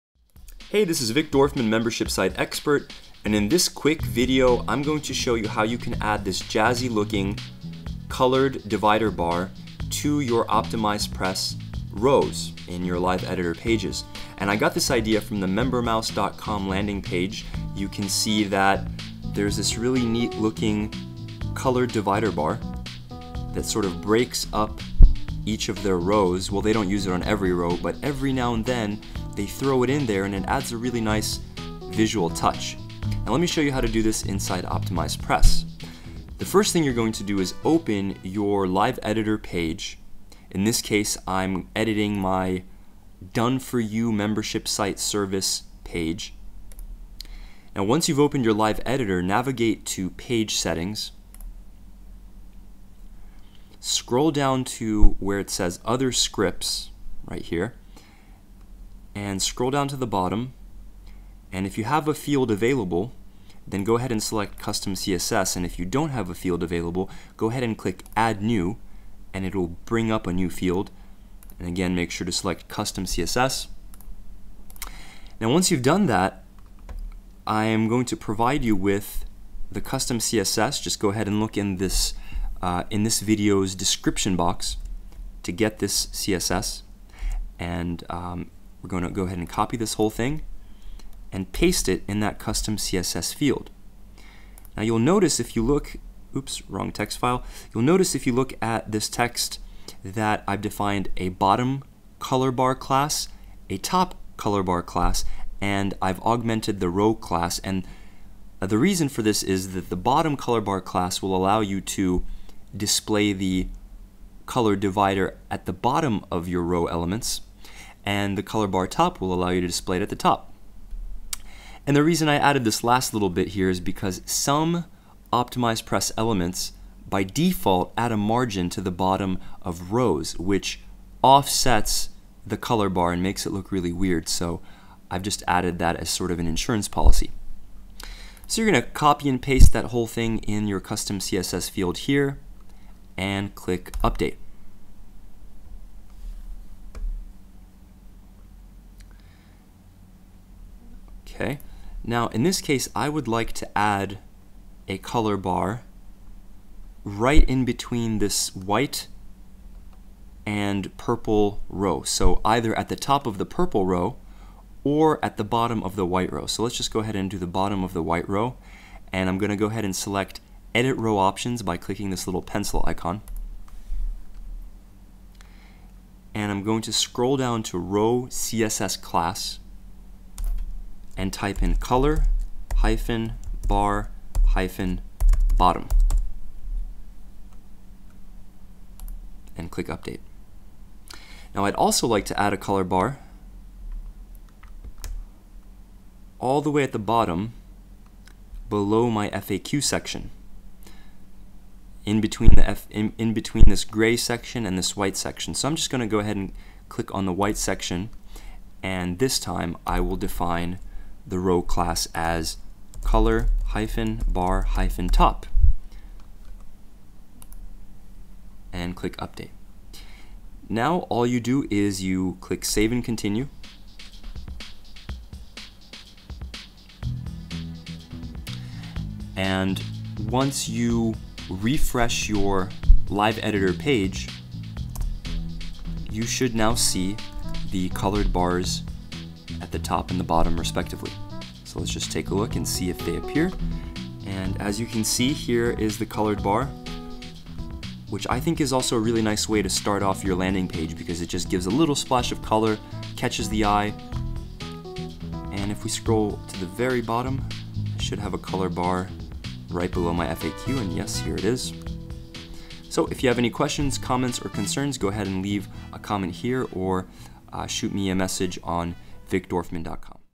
How to add a colored row divider to your OptimizePress rows
In this video I show you how to add a colored row divider to your OptimizePress Live Editor pages.

You can add this divider either on the top of rows, on the bottom of your rows, or both!

Also, the CSS I provided above already has the image in my CloudFront account, so there's no need for you to upload it to your site. :)

However, if you want to use an alternate row divider image you can certainly do that.